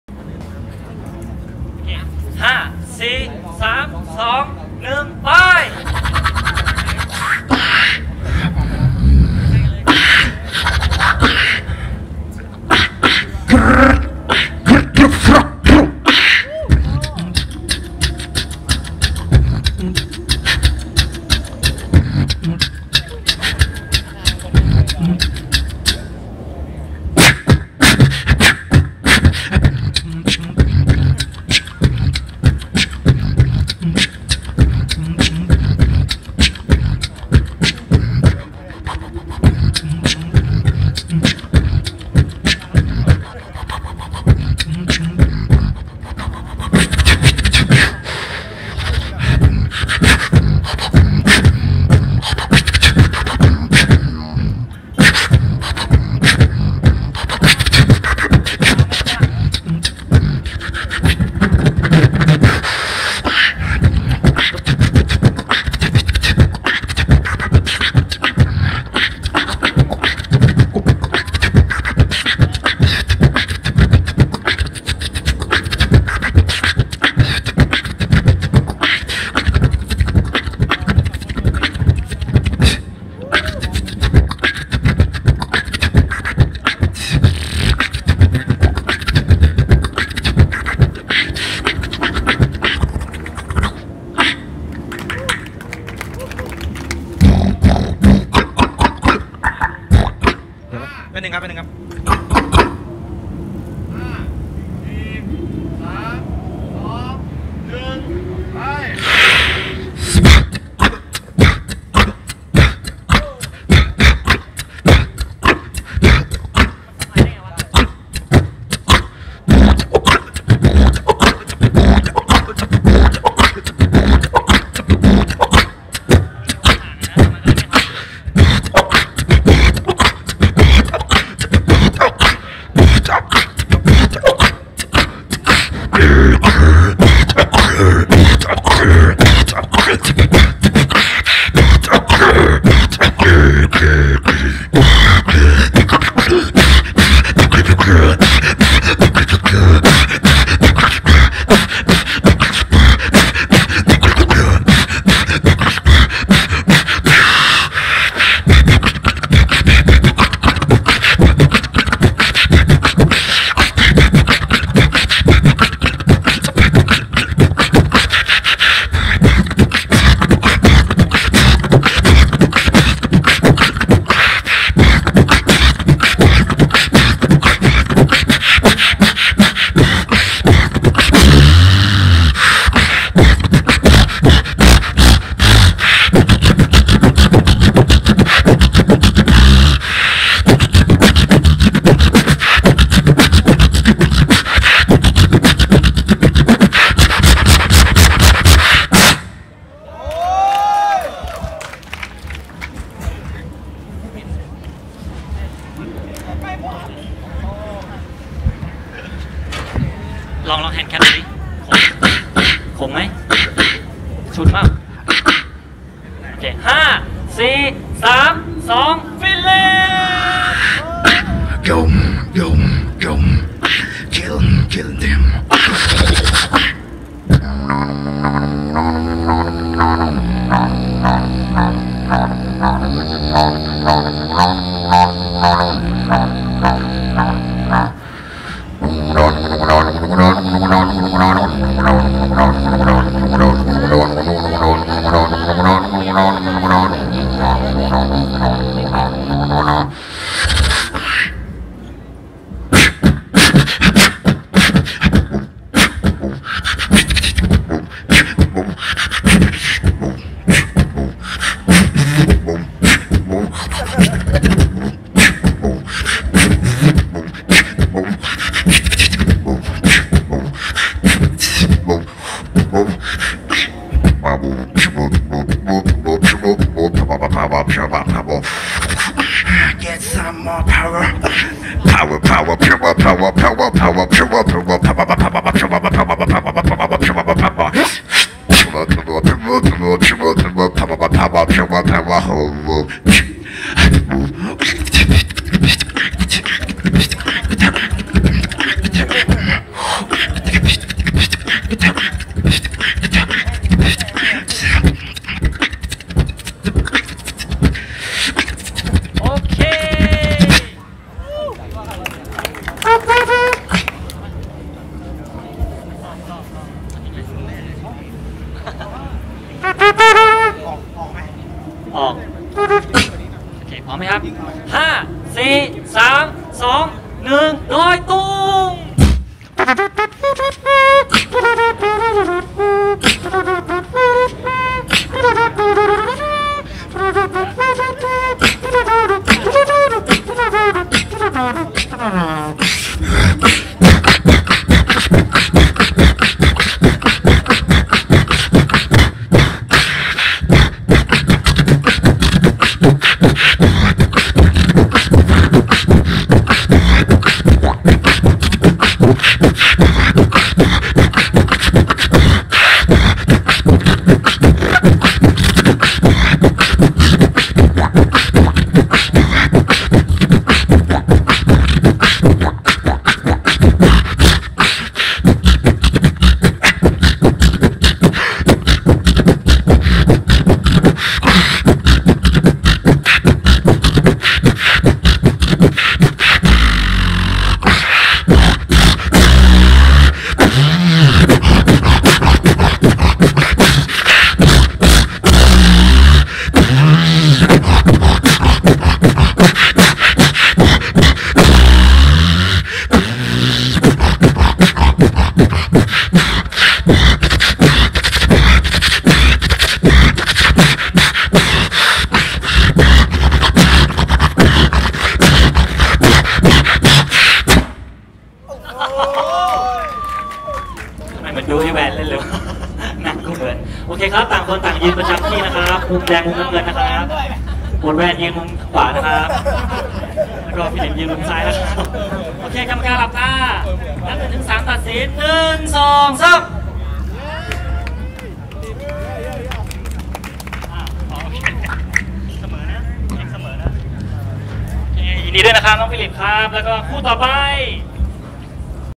Philippe vs Doytung | Quarter Final | K-Battle 2014
Jury: Mit Doubletap, MC Ling, Gurupom

Camera,Audio: Teelit, Duck4real

9/11/2014